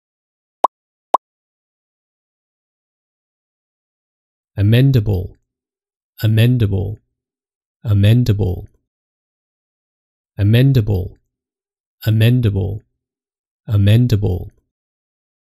How to Pronounce Amendable
In this video we'll show you how to correctly pronounce:  Amendable.